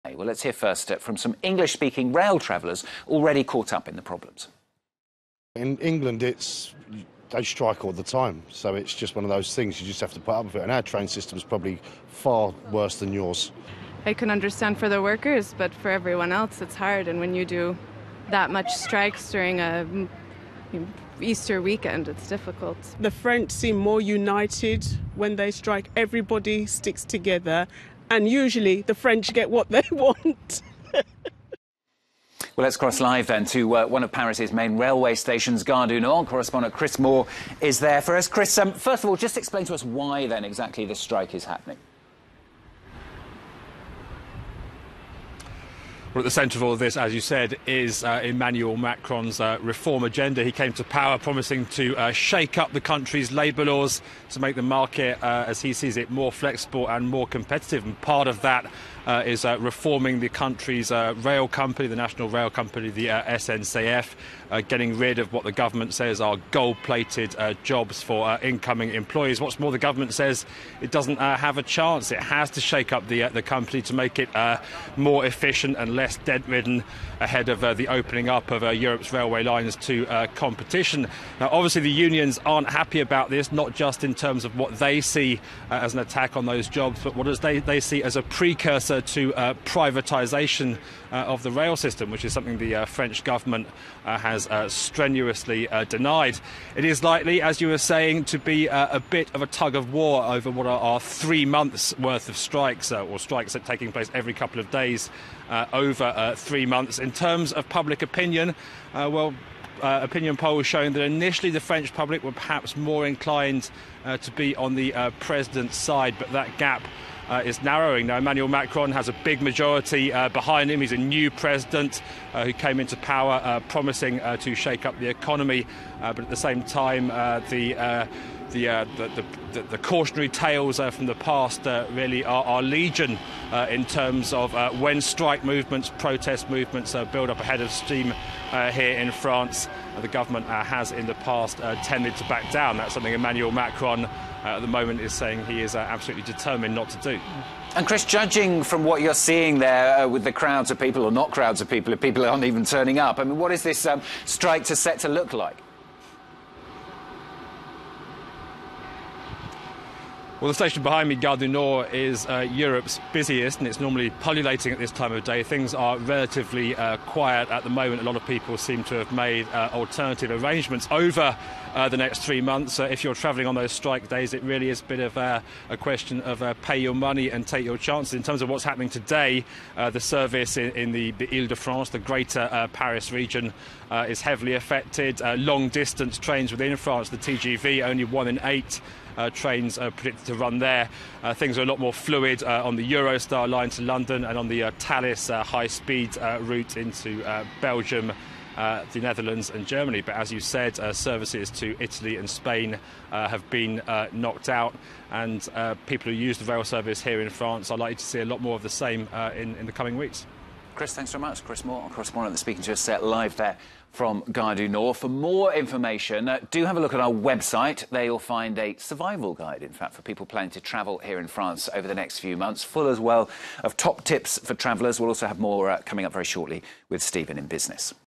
French strikes: A guide for commuters on how to dodge transport strife
French state railway operator SNCF warned Sunday of major disruptions caused by strikes this week that analysts say will be a major test of the political weight of the country's once fearsome trade unions.